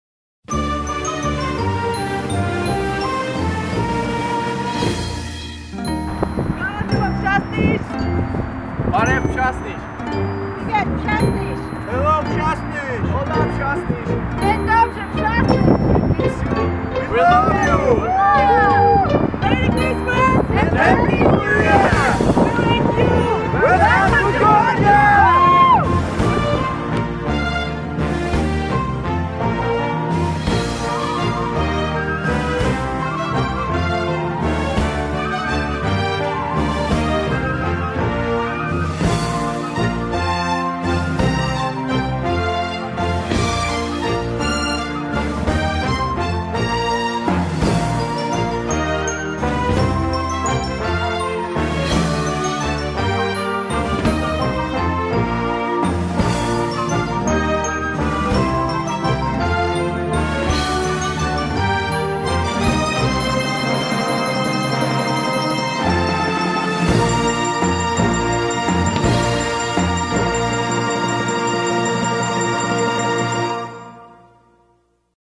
Życzenia z Gruzji :)
Życzenia bożonarodzeniowe i noworoczne od naszych ormiańskich Przyjaciół z Gruzji, którzy gościli w Dekanacie Przasnyskim w czasie ŚDM 2016.